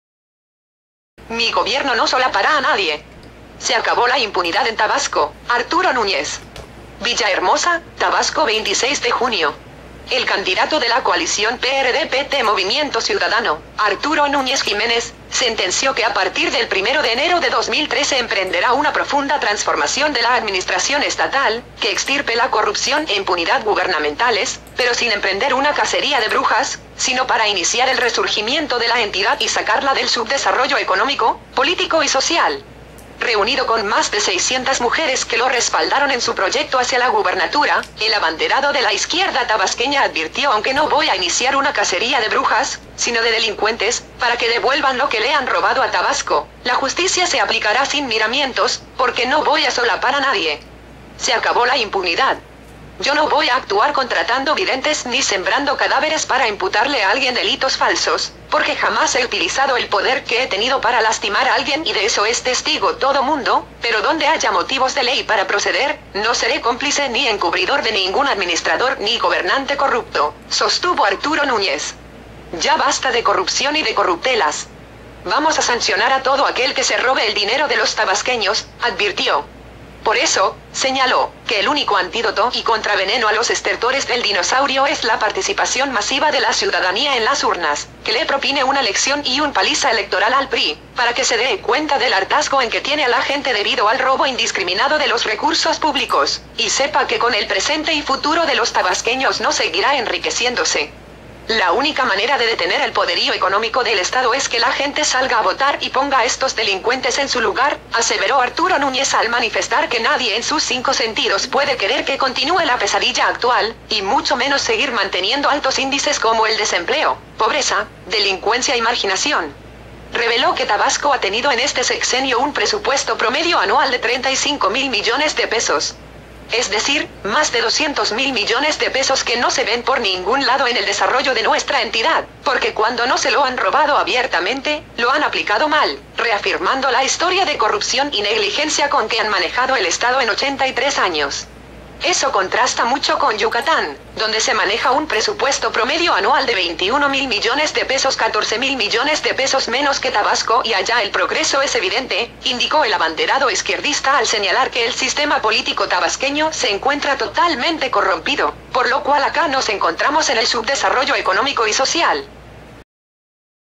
Mi gobierno no solapará a nadie; se acabó la impunidad en Tabasco, Arturo Núñez
Villahermosa, Tabasco 26 de junio.- El candidato de la coalición PRD-PT-Movimiento Ciudadano, Arturo Núñez Jiménez, sentenció que a partir del primero de enero de 2013 emprenderá una profunda transformación de la administración estatal, que extirpe la corrupción e impunidad gubernamentales, pero sin emprender una 'cacería de brujas', sino para iniciar el resurgimiento de la entidad y sacarla del subdesarrollo económico, político y social.

Reunido con más de 600 mujeres que lo respaldaron en su proyecto hacia la gubernatura, el abanderado de la izquierda tabasqueña advirtió aunque "no voy a iniciar una 'cacería de brujas', sino de delincuentes, para que devuelvan lo que le han robado a Tabasco", la justicia se aplicará sin miramientos, porque "no voy a solapar a nadie; se acabó la impunidad".

Yo no voy a actuar contratando videntes ni sembrando cadáveres para imputarle a alguien delitos falsos, porque jamás he utilizado el poder que he tenido para lastimar a alguien "y de eso es testigo todo mundo", pero donde haya motivos de ley para proceder, no seré cómplice ni encubridor de ningún administrador ni gobernante corrupto, sostuvo Arturo Núñez.

Ya basta de corrupción y de corruptelas; vamos a sancionar a todo aquel que se robe el dinero de los tabasqueños, advirtió. Por eso, señaló, que el único antídoto y contraveneno a los estertores del dinosaurio es la participación masiva de la ciudadanía en las urnas, que le propine una lección y un 'paliza electoral' al PRI, para que se dé cuenta del hartazgo en que tiene a la gente debido al robo indiscriminado de los recursos públicos, y sepa que con el presente y futuro de los tabasqueños no seguirá enriqueciéndose.

La única manera de detener el poderío económico del estado es que la gente salga a votar y ponga a estos delincuentes en su lugar, aseveró Arturo Núñez al manifestar que nadie en sus cinco sentidos puede querer que continúe la pesadilla actual, y mucho menos seguir manteniendo altos índices como el desempleo, pobreza, delincuencia y marginación.

Reveló que Tabasco ha tenido en este sexenio un presupuesto promedio anual de 35 mil millones de pesos; es decir, más de 200 mil millones de pesos que no se ven por ningún lado en el desarrollo de nuestra entidad, porque "cuando no se lo han robado abiertamente, lo han aplicado mal", reafirmando la historia de corrupción y negligencia con que han manejado el estado en 83 años.

Eso contrasta mucho con Yucatán, donde se maneja un presupuesto promedio anual de 21 mil millones de pesos --14 mil millones de pesos menos que Tabasco-- y allá el progreso es evidente, indicó el abanderado izquierdista al señalar que el sistema político tabasqueño se encuentra totalmente corrompido, por lo cual acá nos encontramos en el subdesarrollo económico y social.